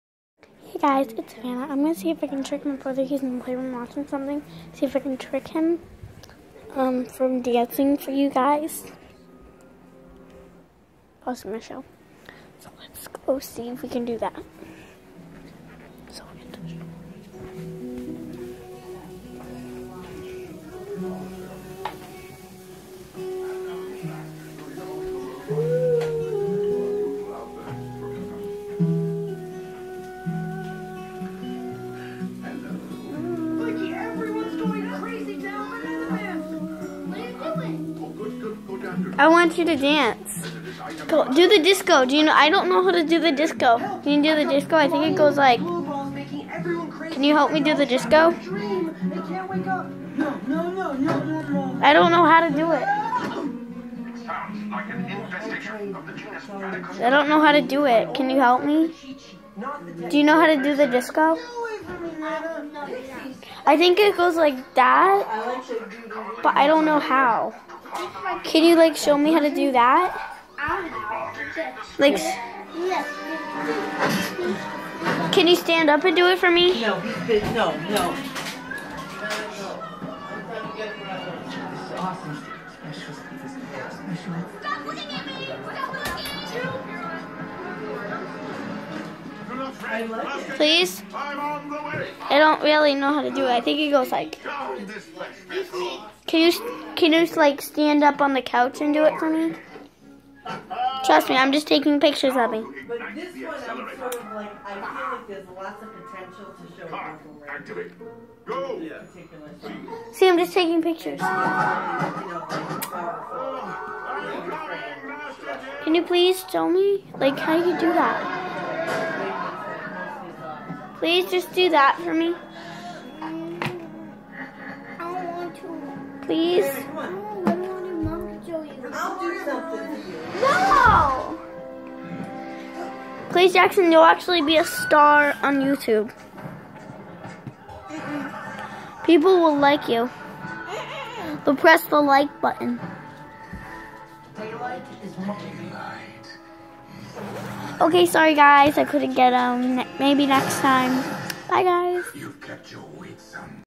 Me trying to get my little brother to do the Disko
Trying to get my little brother to do the Disko it was a fail